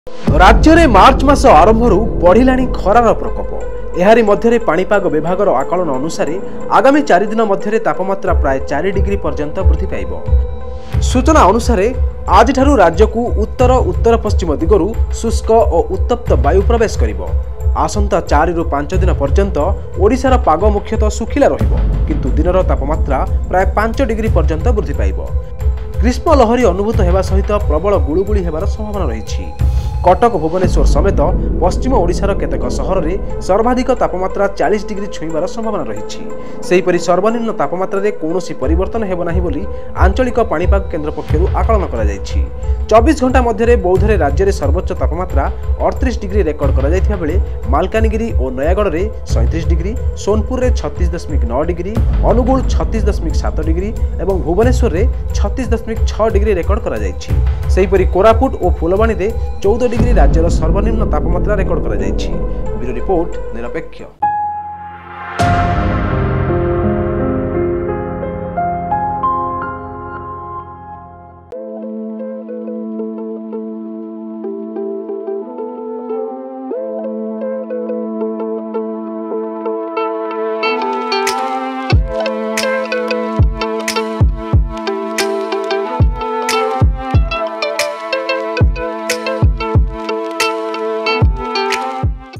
Weather News : Odisha Will Be Hottest from This Month of March || Nirapekshya News
Weather News : Odisha Will Be Hottest from This Month March
🔆MORE UDATE🔆